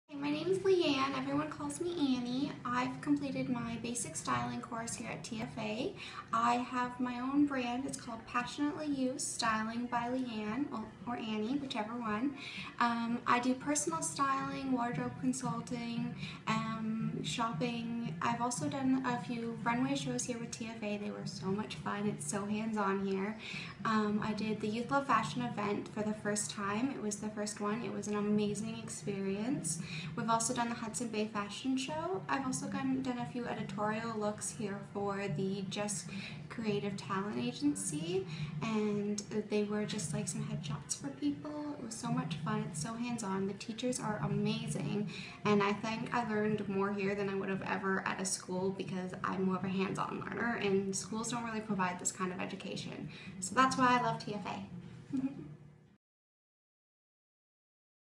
Toronto Fashion Academy: Fashion Styling Student Testimonial
Toronto Fashion Academy's Fashion Styling Testimonial by:

Leanne Mercer
Brand: Passionately You

Portfolio Work:

- Hudson's Bay Wish List 2014 Fashion Show
- Youth Love Fashion 2014 Fashion Show
- Portfolio Shoots for several Agencies & Models